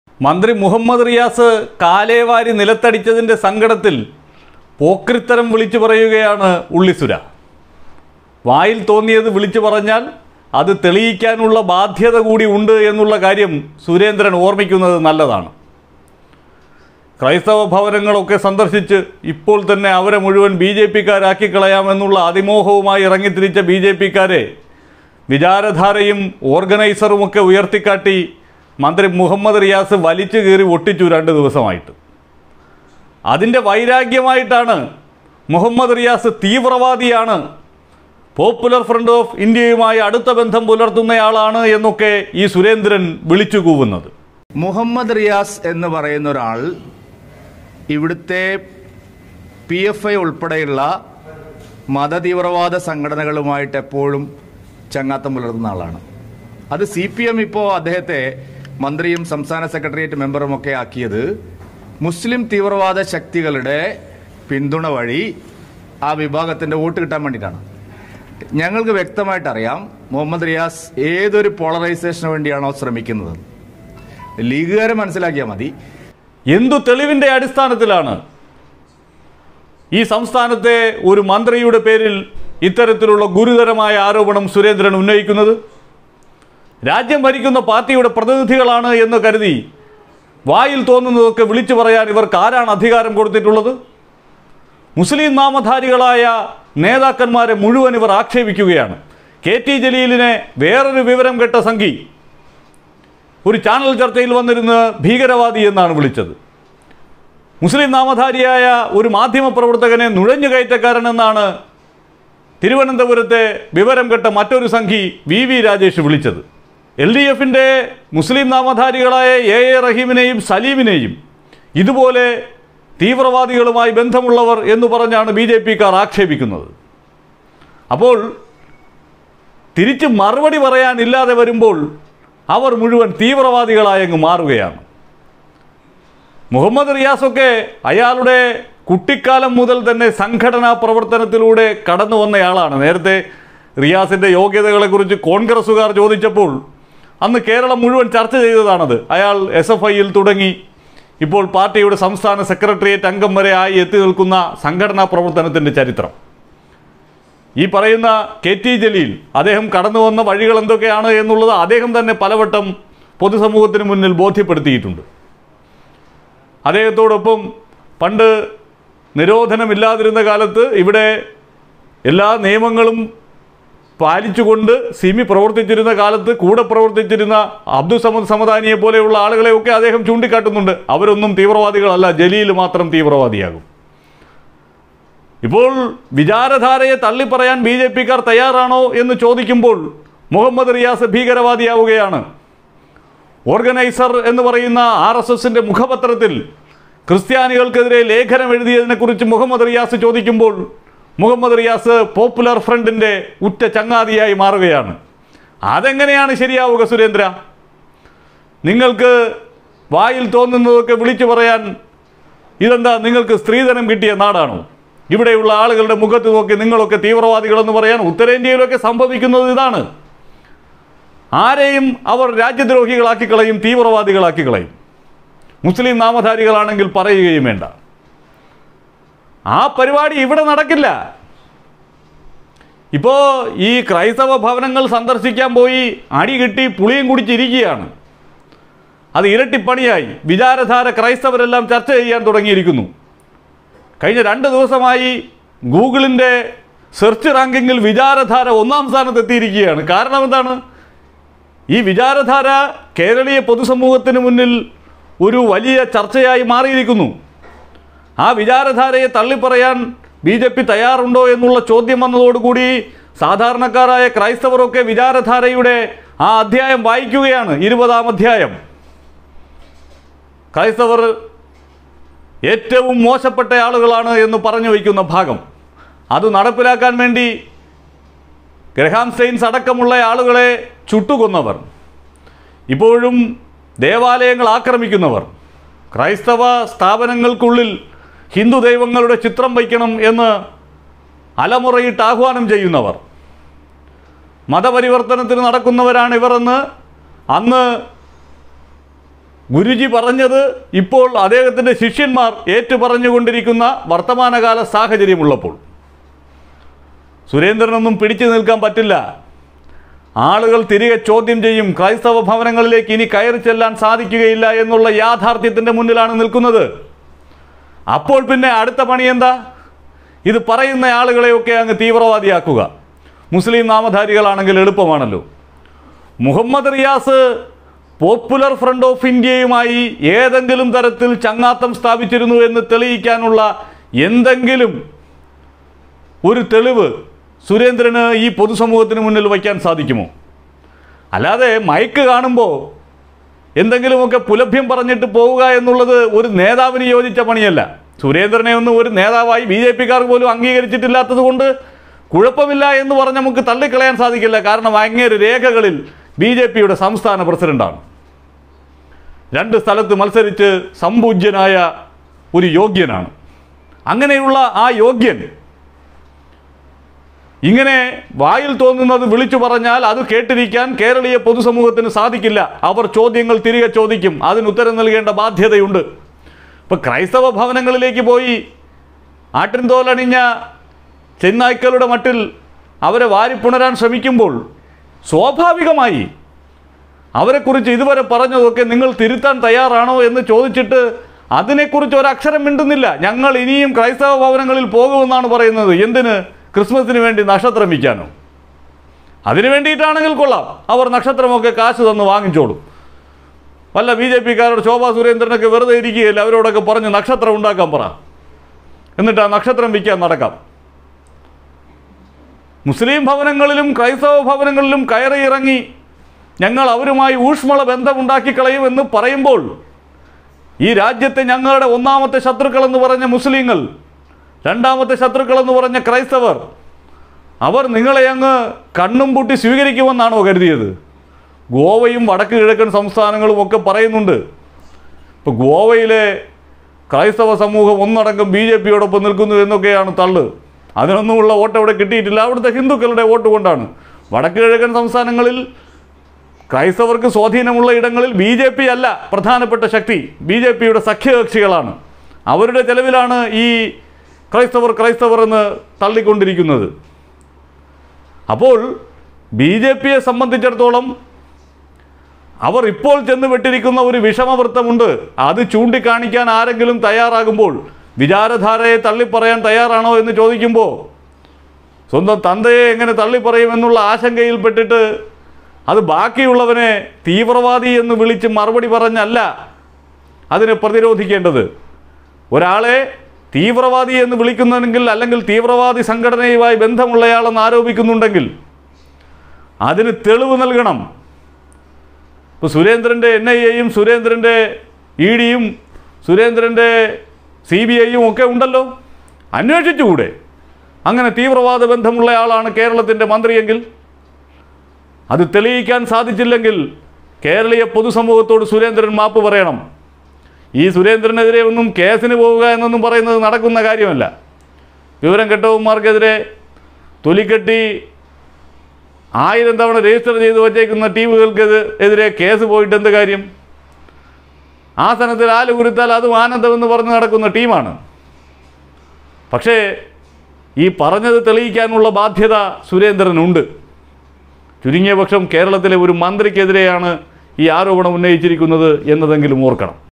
അയ്യയ്യേ...സുരേന്ദ്രന്‍ എന്തിനാണിങ്ങനെ എന്നും പരിഹാസ്യനാവുന്നത്... | K Srendran | Muhammad Riyas
ഉള്ളിയില്ലാതെ കരയേണ്ടി വരുമല്ലോ സുരേ

റിയാസ് കാലേ വാരി നിലത്തടിച്ചതിന്
പോക്രിത്തരം പറഞ്ഞാലെങ്ങനെ സുരേ